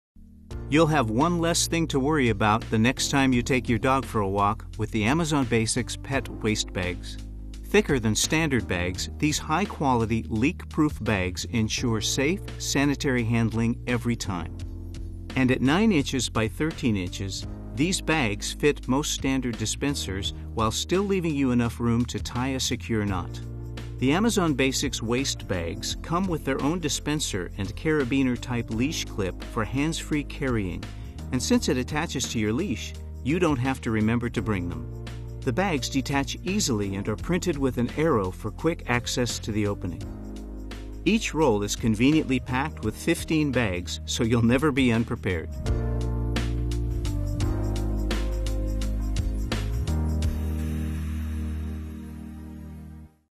AmazonBasics-Dog-Waste-Bags-with-Dispenser-and-Leash-Clip---600-Count - Amazon
+ 600 black dog-waste bags and dispenser with leash clip
+ Measures approximately 13x9 inches (LxW)
+ Leak-proof bags for safe, sanitary handling; arrows point to the bag opening for quick access
+ 15 bags per roll (40 total rolls); the bags are thicker than standard 20 bag per roll designs
+ Rolls fit standard-size leash dispensers and quickly pull out